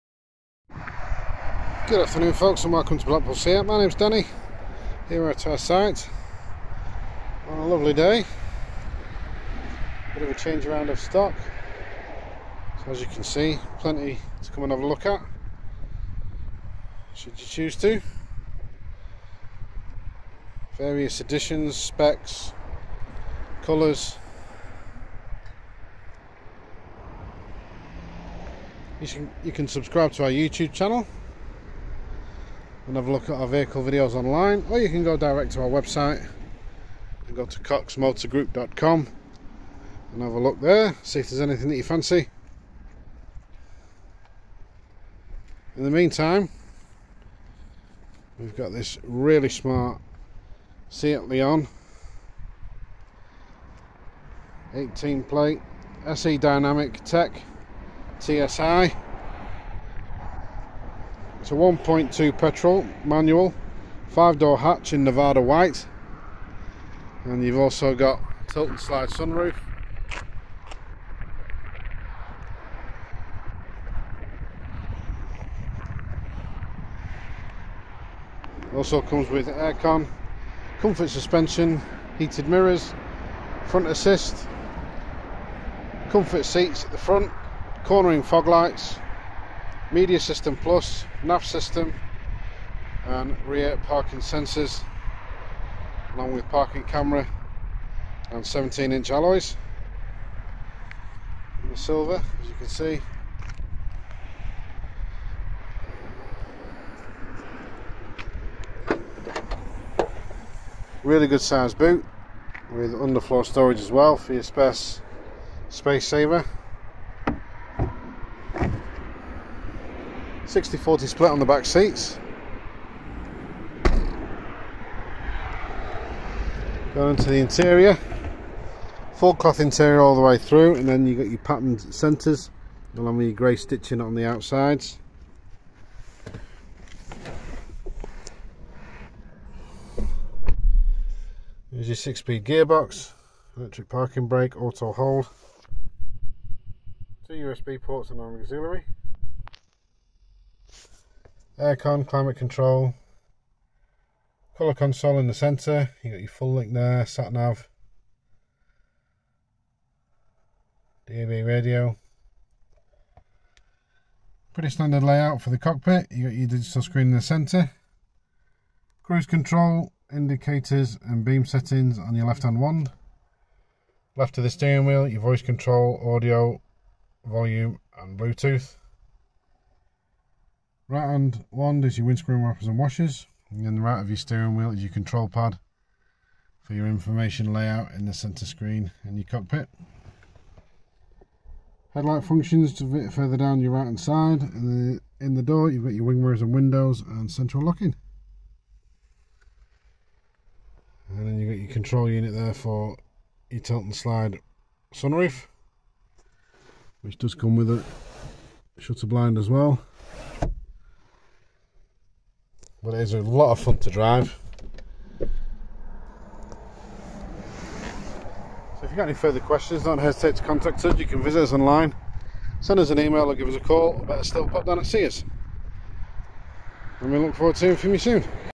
SEAT 2018 LEON SE DYNAMIC TECHNOLOGY TSI | BLACKPOOL SEAT
SEAT 2018 LEON SE DYNAMIC TECHNOLOGY TSI. 1.2 Petrol 6 Speed Manual. 5 Door Hatch. "Nevada White" Metallic.
Vehicle Features are:
Tilt n Slide Sunroof
Air Con
Comfort Suspension
Heated Wing Mirrors
Front Assist
Comfort Seats
Media System+
NAV System